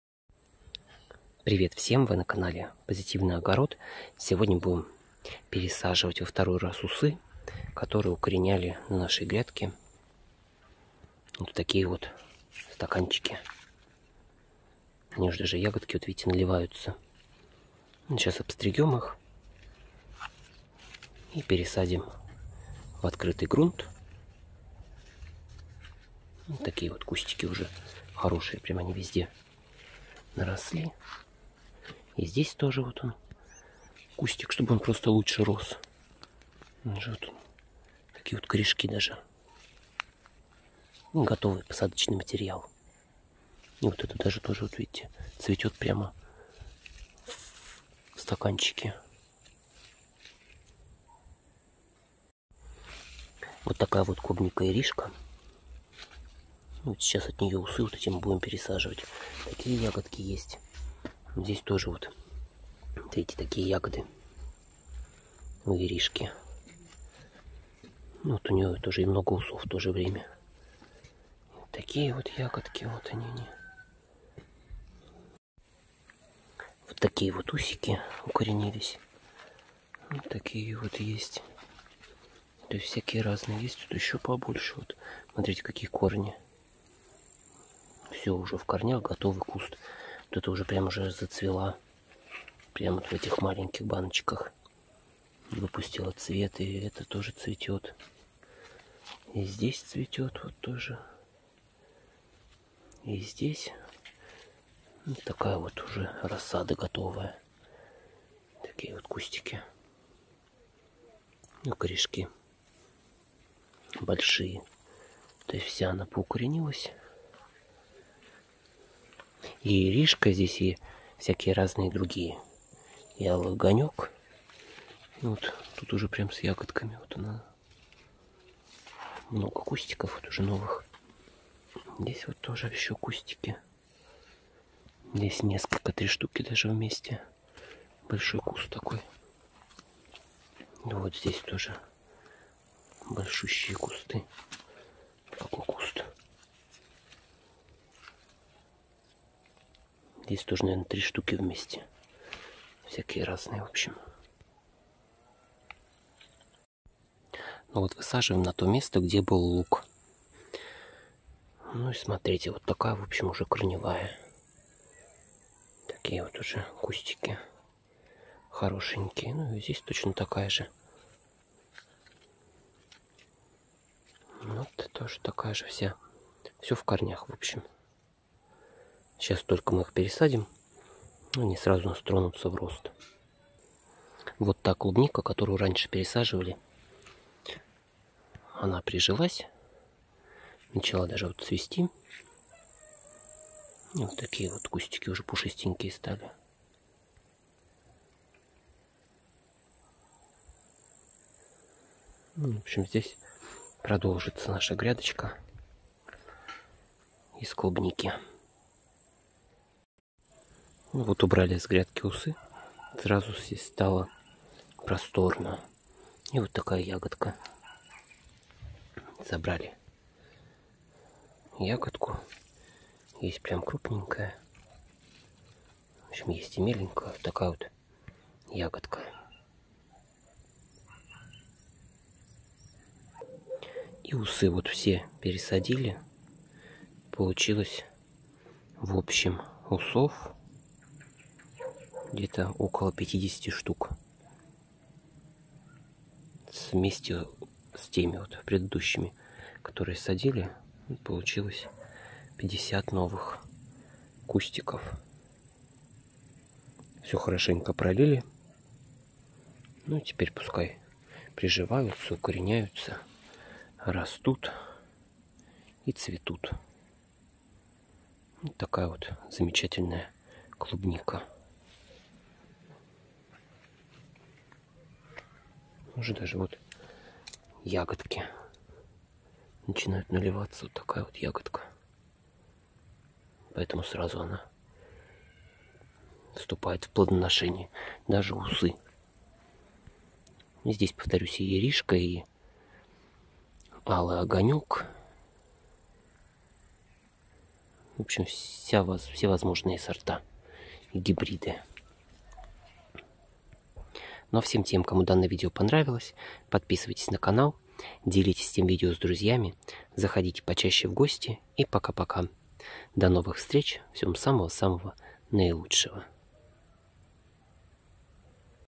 Размножение клубники усами в стаканчиках и их пересадка на грядку
Пересадка укоренившихся в стаканчиках усов клубники в августе на временную грядку.